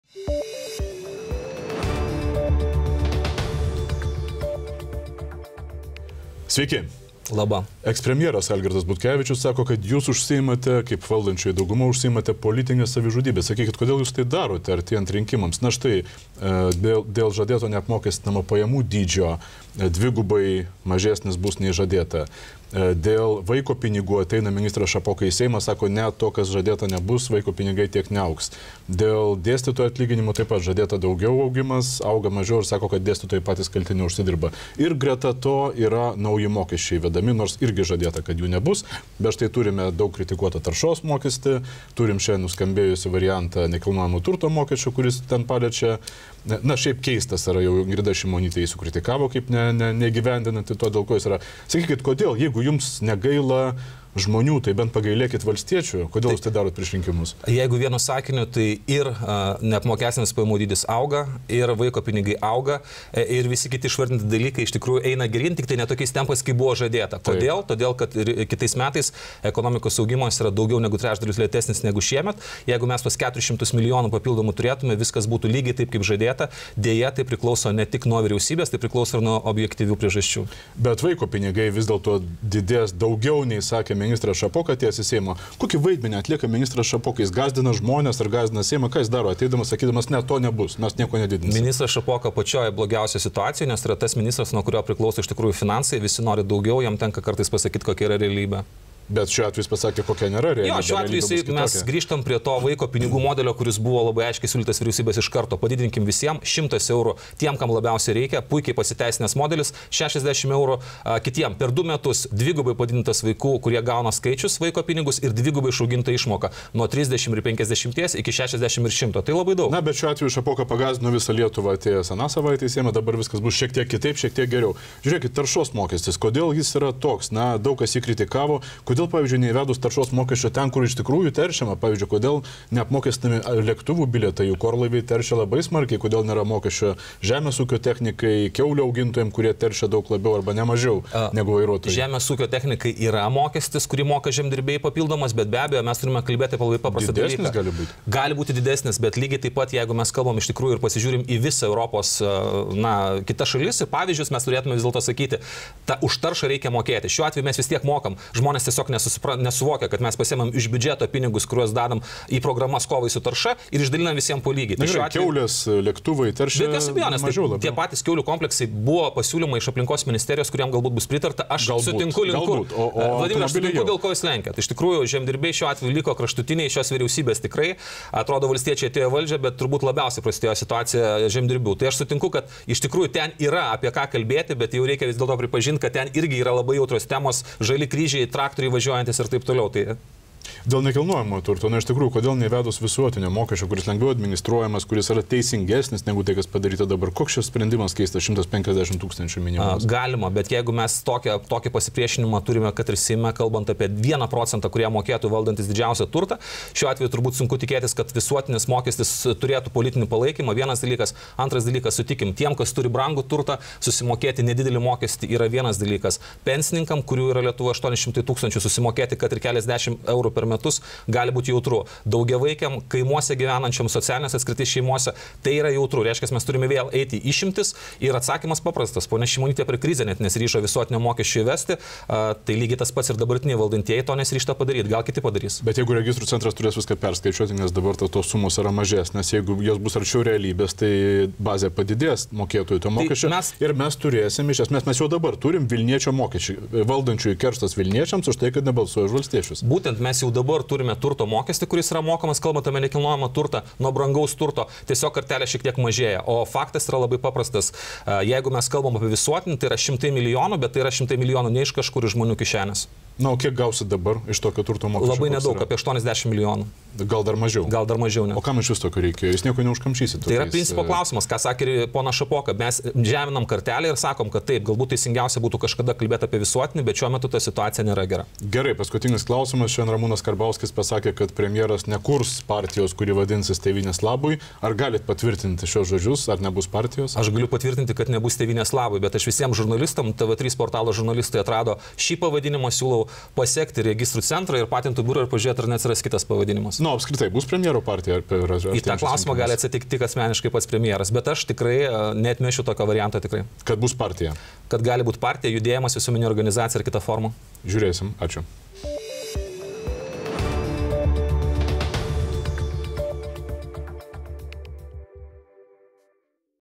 Malinauskas: visi nori daugiau, o Šapokai tenka pasakyti, kokia yra realybė
Ar Sauliaus Skvernelio vadovaujama Vyriausybė stengiasi, kad valstiečiai kuo skaudžiau pralaimėtų artėjančius Seimo rinkimus? Valdantieji atšaukia visuomenei anksčiau duotus pažadus ir skubiai įvedinėja naujus, daugelio kritikuojamus mokesčius.
Kas lemia jų keistus sprendimus klausiame premjero patarėjo Skirmanto Malinausko.